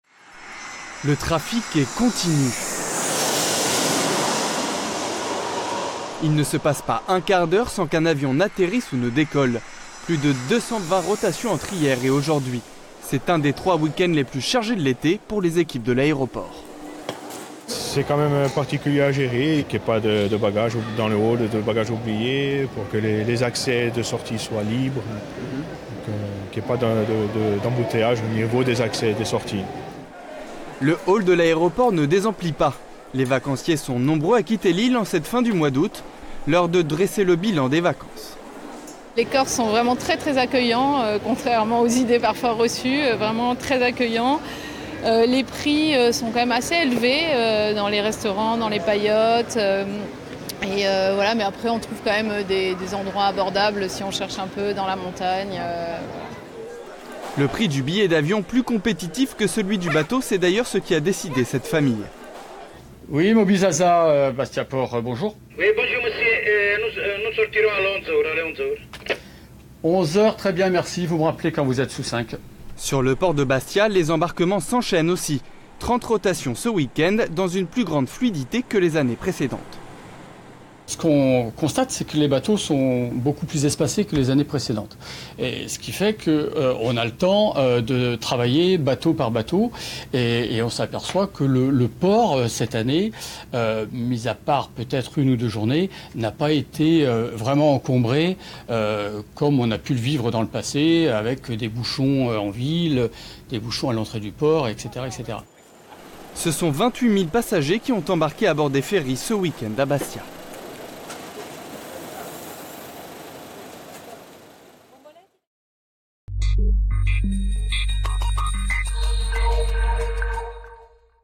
Corse : la fin des vacances a sonné pour des milliers de touristes
Ce week-end, les ports et les aéroports de Corse n’ont pas désempli. Pour des milliers de touristes, il est temps de reprendre le travail. Illustration à Bastia.

Intervenants - Hervé Fini, Chef d'équipe à l'aéroport de Bastia-Poretta ; Pierre-Yves Olivier, Officier de port à la capitainerie de Bastia.
Equipe - Grégoire Alcalay ; Christian Giugliano ; Christophe Gineste.